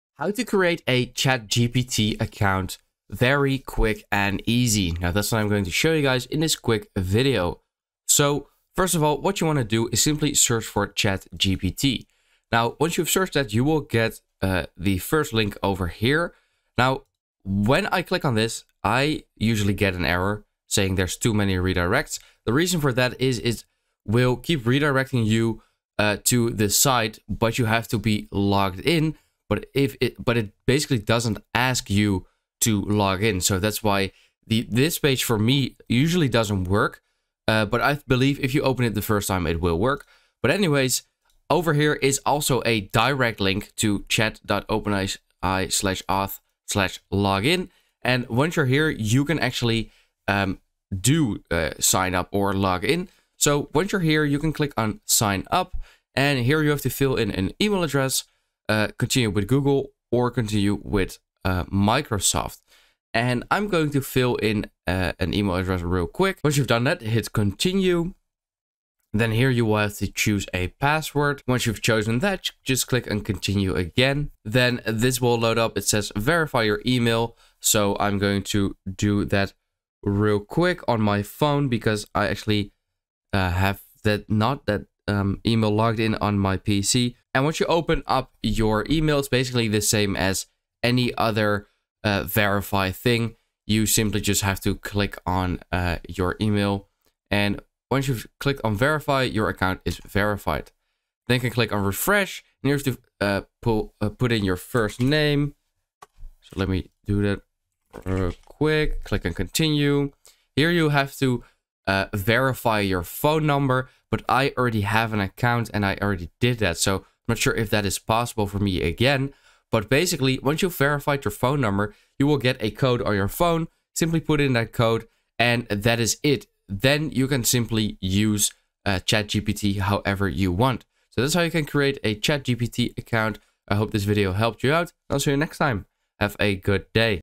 How to Create a ChatGPT Account (Quick & Easy)
In this video I show you how to Create a ChatGPT Account very Quick and Easy

Do you want to know how to Create a ChatGPT Account very Quick and Easy Then I'll show you how to make a ChatGPT Account in 2025

This is a step by step OpenAI tutorial on how to sign up for ChatGPT very easy.